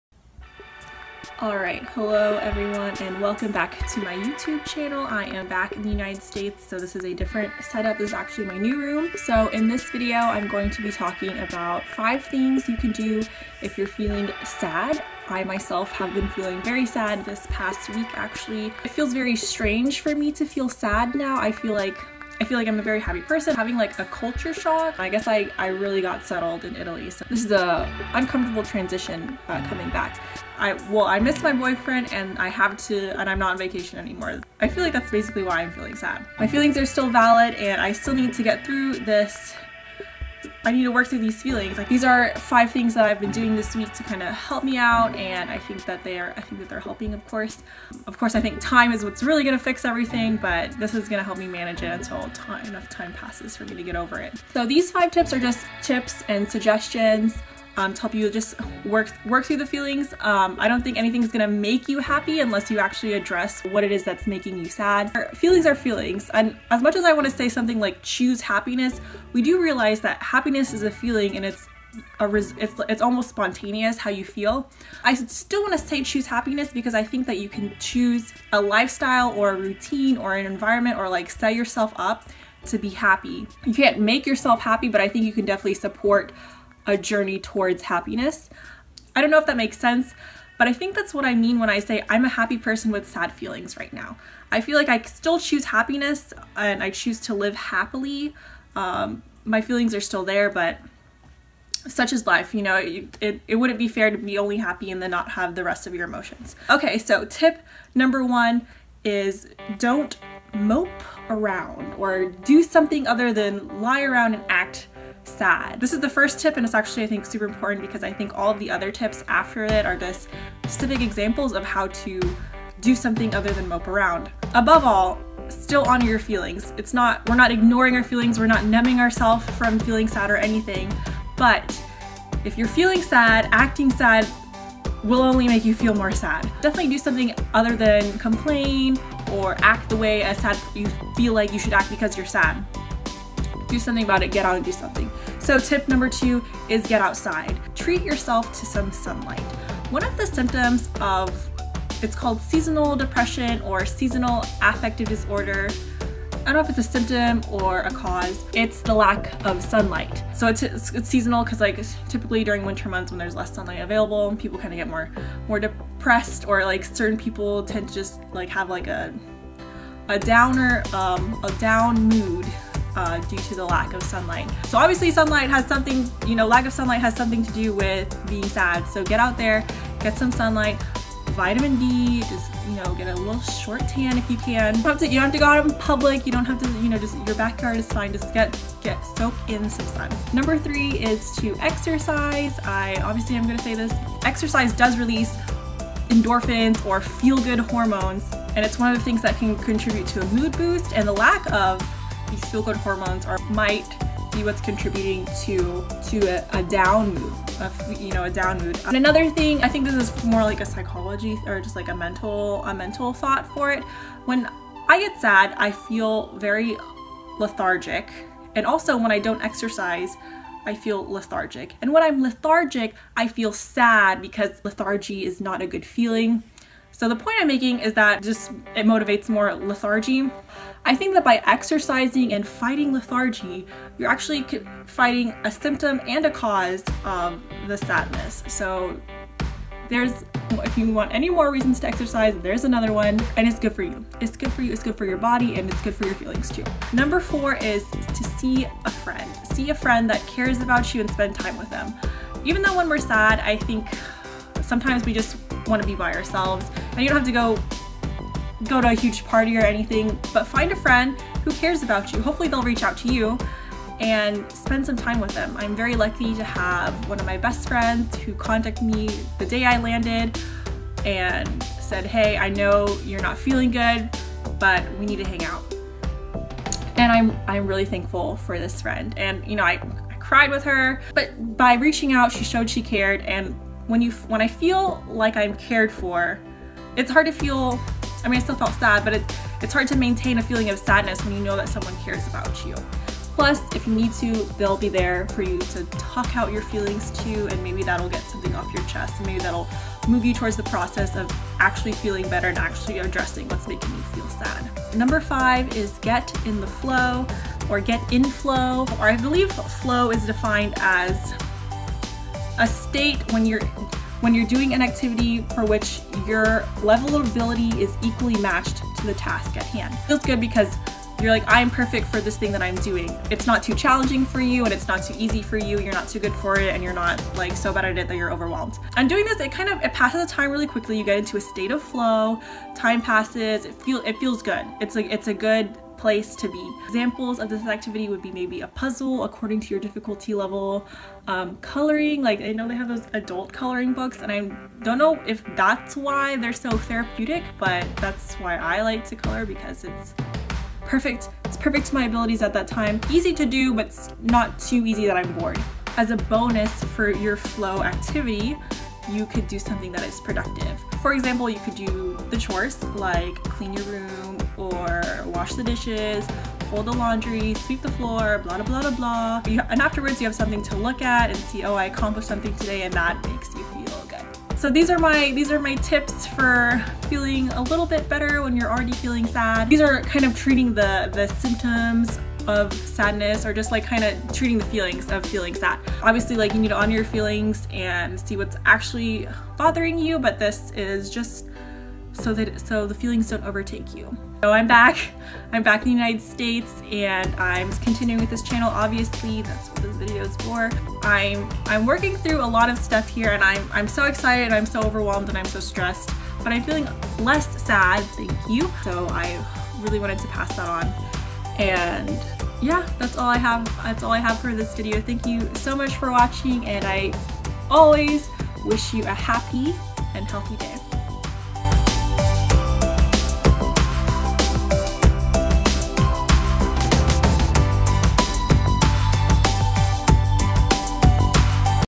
Getting Through Sad Feelings | prudenfox
Hello YouTube and friends!

I'm back in the USA and I've actually been feeling pretty sad lately. Not a normal feeling, but I still strive to "live like I'm happy" in order to support those feelings. At the very least, I sought to do these 5 things to keep myself going and avoid perpetuating the unhappiness. There are lots of other things I could've done, but I felt these tips were easy to execute so I was able to force myself to keep these up.

This video is part of the "Health & Wellness" videos I'm making for this channel. As exemplified in this video, I want to give attention to Mind and Spirit (non-physical) health as opposed to solely physical health/fitness.

Even so, I'm still exploring content and topics AND working on my video skills. This video features more music than normal.

Likes are also a great and super easy way to offer feedback :)